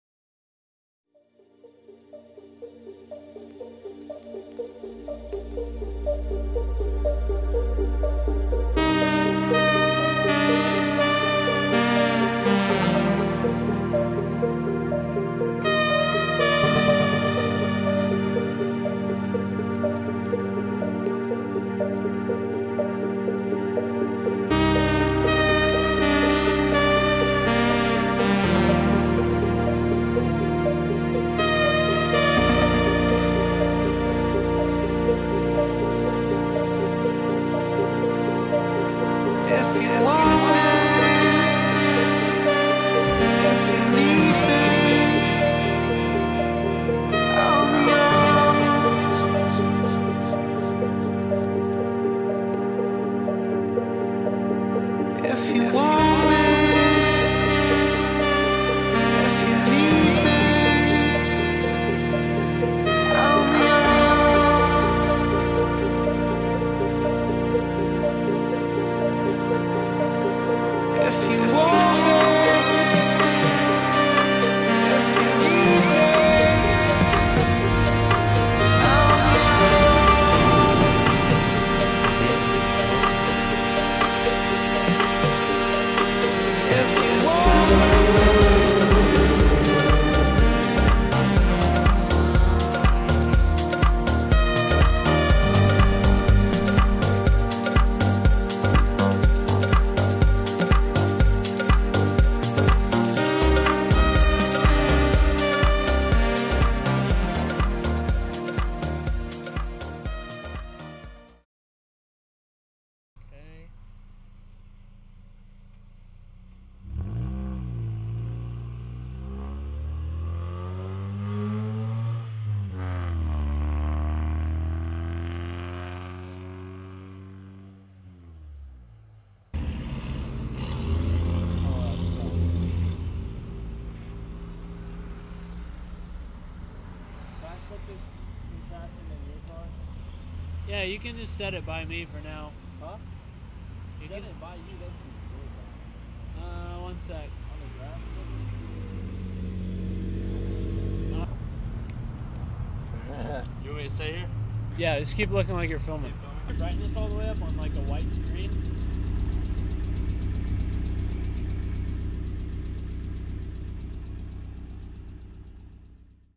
Late Night Drive Miata - Short Film // IN 4K
thank you miles for letting me be your friend and filming your Miata for this short film! enjoy!!